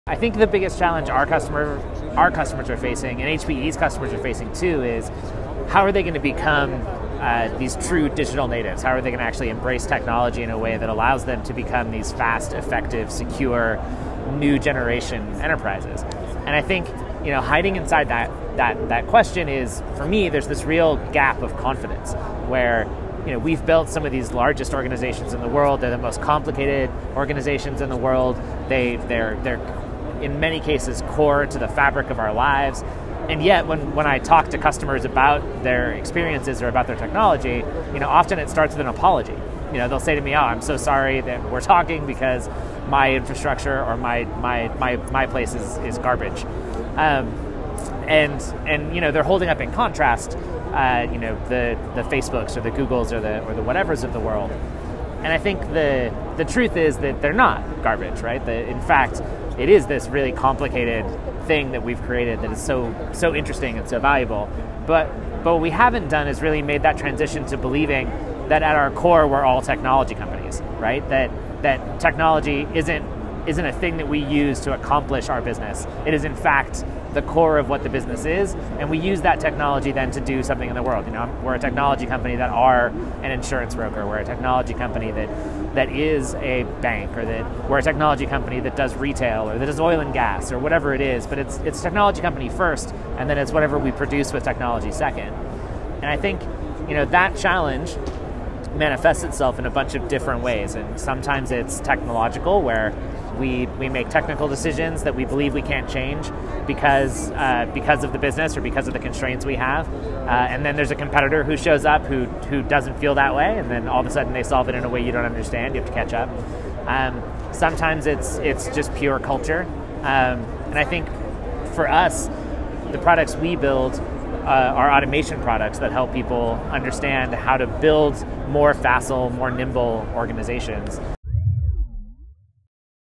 Adam Jacob, CTO at Chef (Part 1)
Many businesses are facing a "gap of confidence" when it comes to embracing Digital Transformation says Chef CTO Adam Jacob.